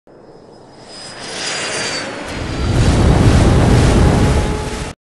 water explosion
still in progress
software use
3ds max particles
fusion